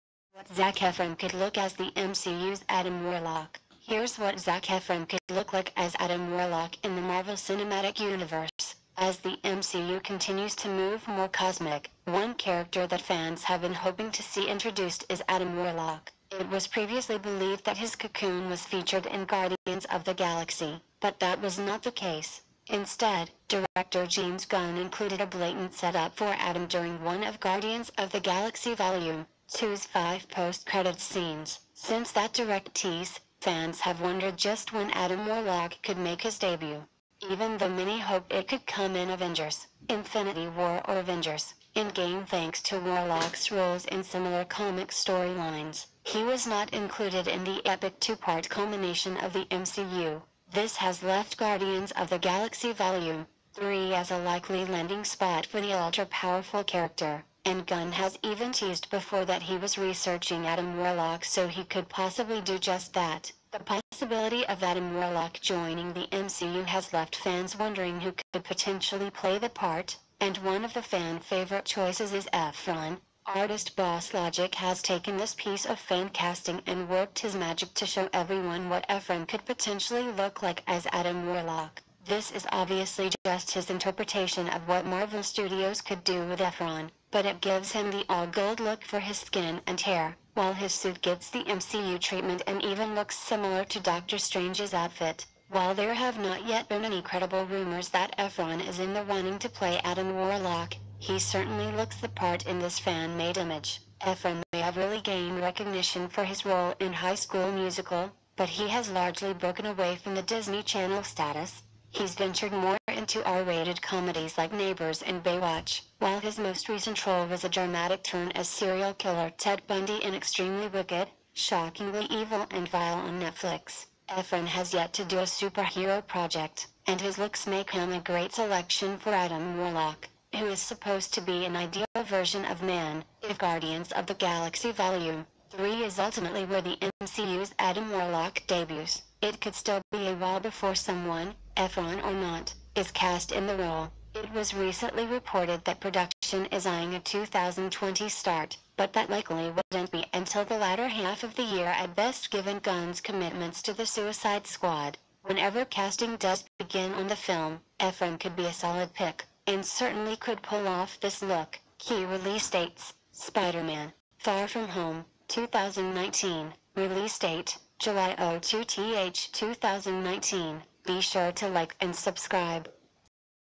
Here's What Zac Efron Could Look As The MCU's Adam Warlock!
While there have not yet been any credible rumors that Efron is in the running to play Adam Warlock, he certainly looks the part in this fan-made image. Efron may have really gained recognition for his role in High School Musical, but he has largely broken away from the Disney Channel status. He's ventured more into R-rated comedies like Neighbors and Baywatch, while his most recent role was a dramatic turn as serial killer Ted Bundy in Extremely Wicked, Shockingly Evil and Vile on Netflix. Efron has yet to do a superhero project, and his looks make him a great selection for Adam Warlock, who is supposed to be an ideal version of man.

If Guardians of the Galaxy Vol. 3 is ultimately where the MCU's Adam Warlock debuts, it could still be a while before someone - Efron or not - is cast in the role. It was recently reported that production is eyeing a 2020 start, but that likely wouldn't be until the latter half of the year at best given Gunn's commitments to The Suicide Squad. Whenever casting does begin on the film, Efron could be a solid pick - and certainly could pull off this look.

KEY RELEASE DATES
Spider-Man: Far From Home (2019) release date: Jul 02, 2019